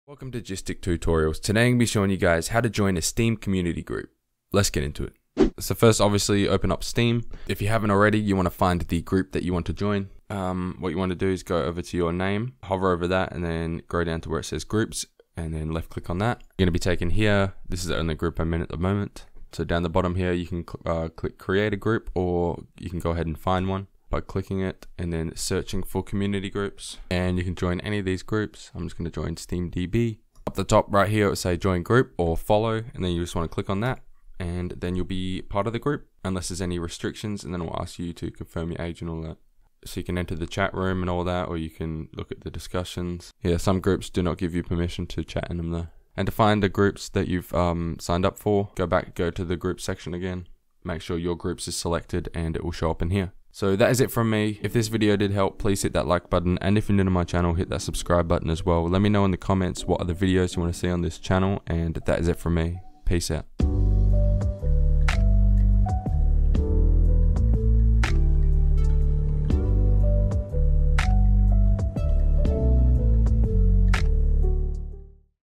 How To Join A Steam Community Group
Chapters:
Intro ➤ 0:00-0:05
Find A Community Group ➤ 0:05-0:30
Joining Group ➤ 0:30-0:45
Locating Your Groups ➤ 0:45-0:53
Outro ➤ 0:53-

~ GysticTutorials